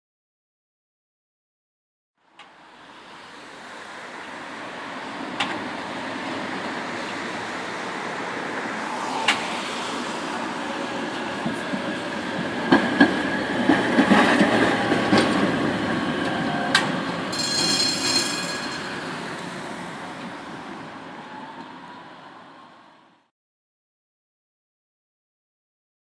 Tatra T4 Schienenschleifwagen (201 002) @ Dresden-Schäferstraße
Durchfahrt des Schienenschleifwagens der DVB (Dresdner Verkehrsbetriebe) durch die Haltestelle 'Schäferstraße' am 19. März 2013.